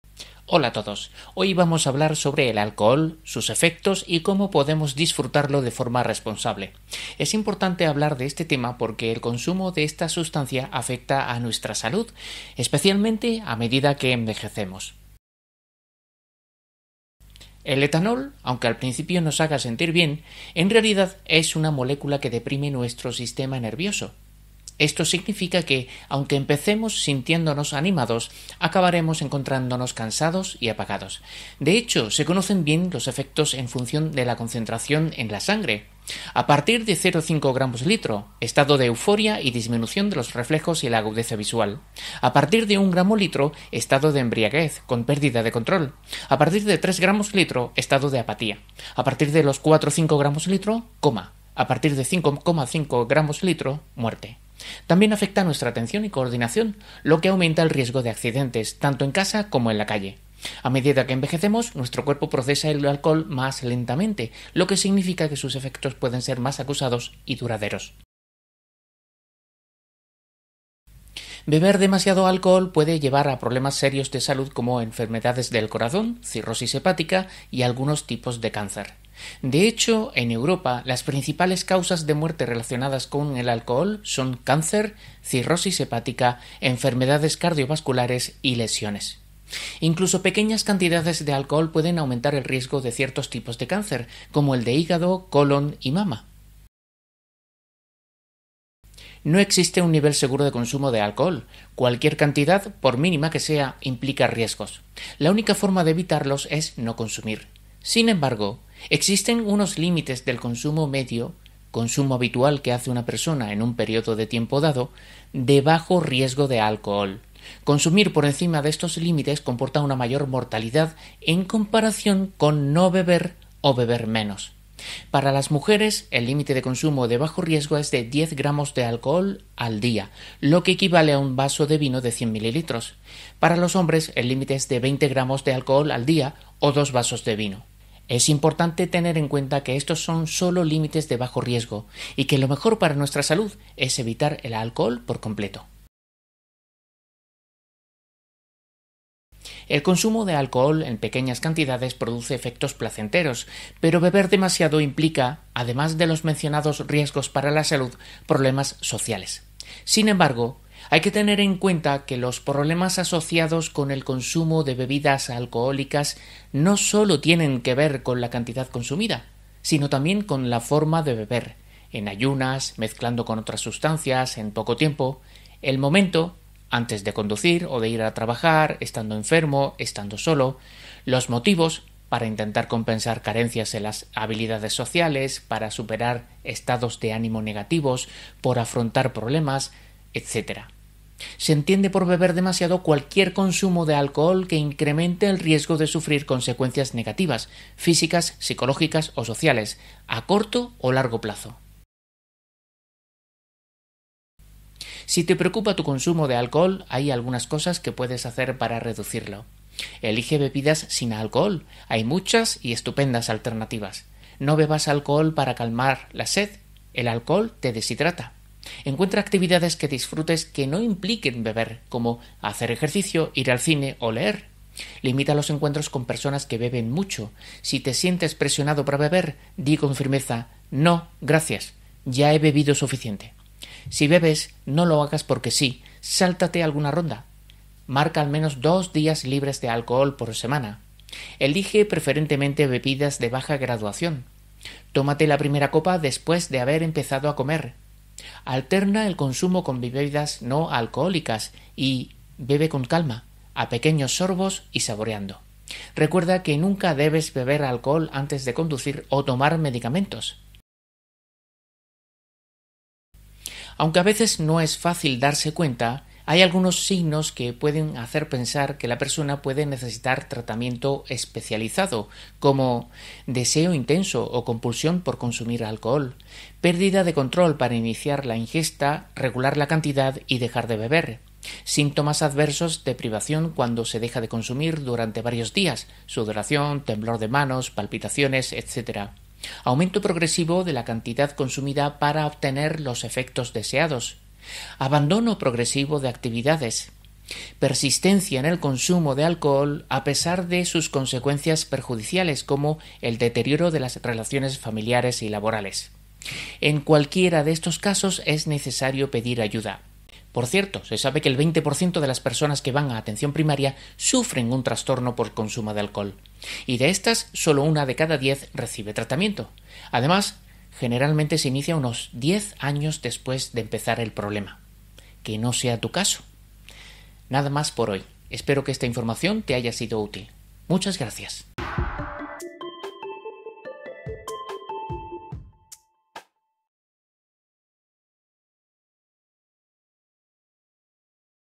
Alcohol ‐ alcohol
En este vídeo hablamos sobre el consumo de alcohol y sus riesgos para la salud. Se exponen las consecuencias del alcoholismo, incluyendo su impacto en la mortalidad y la incidencia de diversas enfermedades como el cáncer. También se proporcionan recomendaciones para prevenir el trastorno por consumo de alcohol, incluyendo límites de consumo seguro y consejos prácticos para reducir el consumo. Finalmente, se menciona cuándo es necesario buscar ayuda profesional para tratar el alcoholismo, describiendo signos que pueden indicar la necesidad de intervención médica.

0:00 Introducción
0:16 Efectos
1:09 Riesgos del consumo excesivo
1:37 Límites de consumo
2:27 Prevención del trastorno por consumo de alcohol
3:22 Consejos para reducir el consumo
4:21 ¿Cuándo es necesario pedir ayuda?